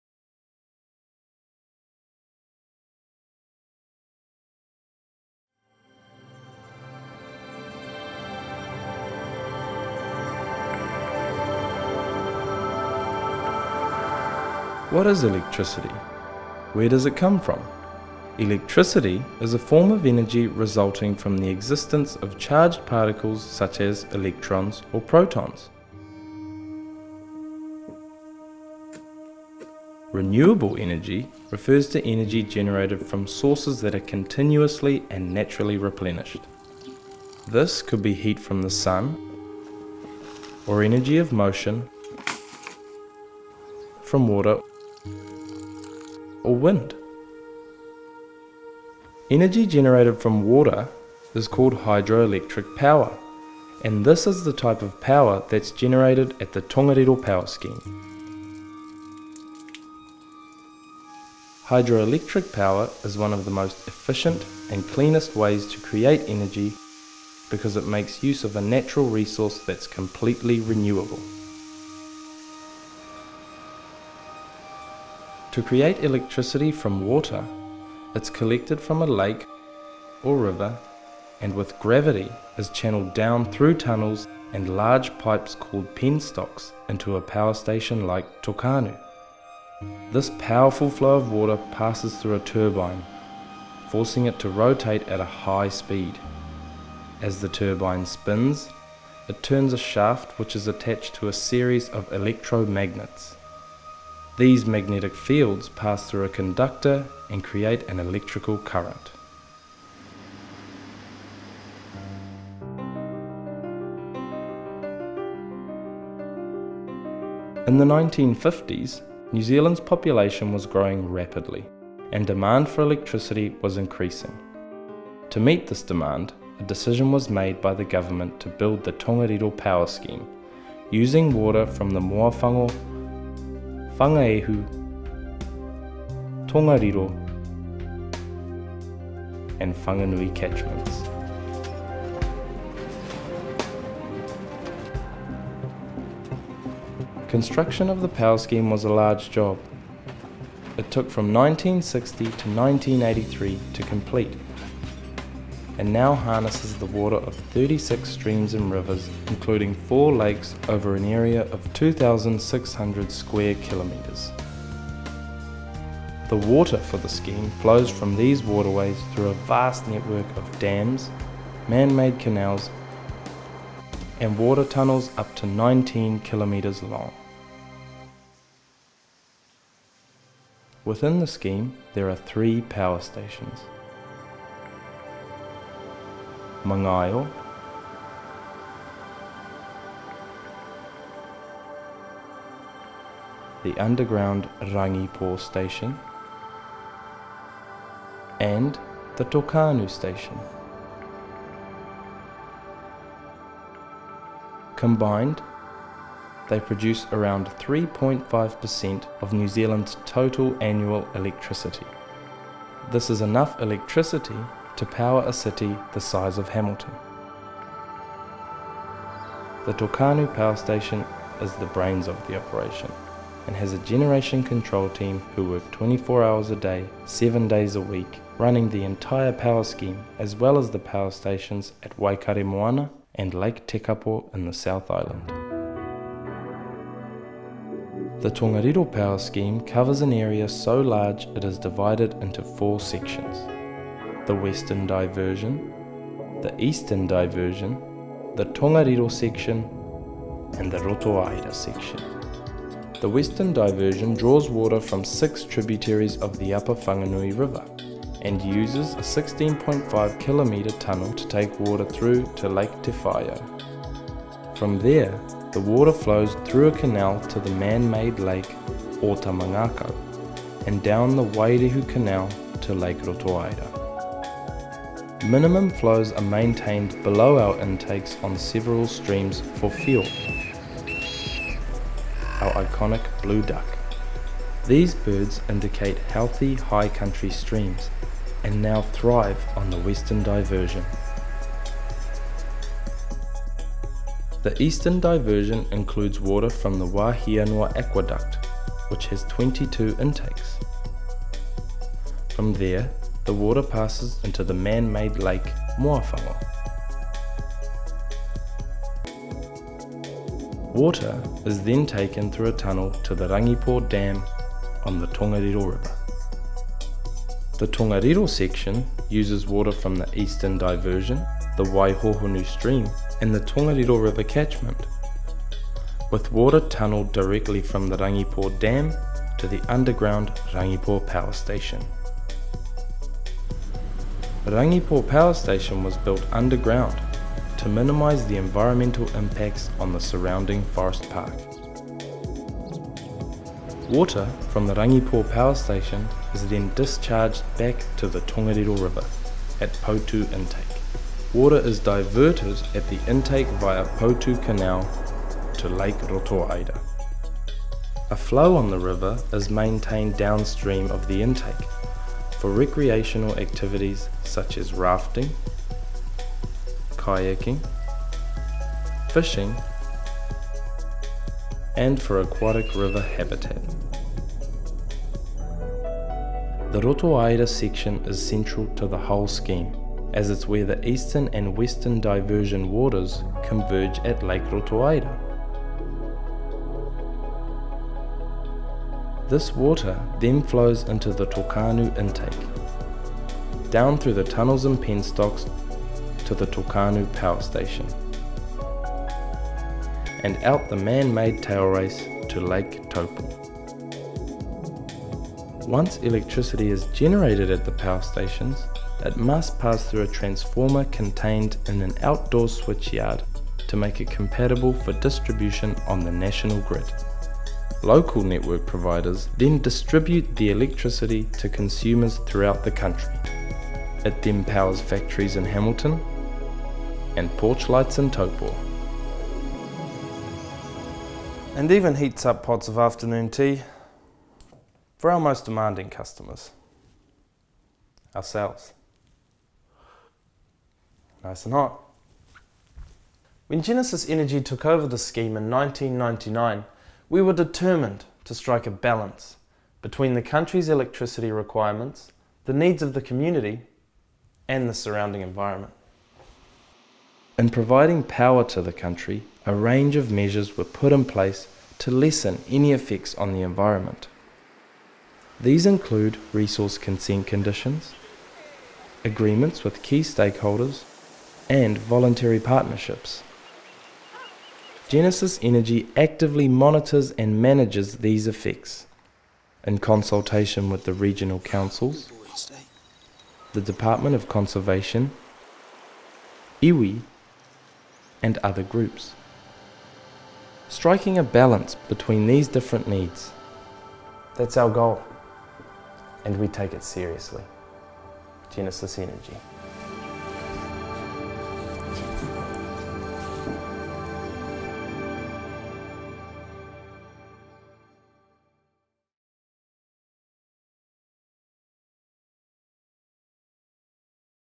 New Zealand Hydro Electric Power
In the 1950s New Zealand's population grew rapidly and demand for electricity was increasing. The decision was made to start the Tongariro Power Scheme to meet this demand. The scheme created 3 power stations; Tokannu, Rangipo and Mangaio. Together they make up 3.5% of New Zealand's total annual electricity.

To learn more about New Zealand Hydro Electric Power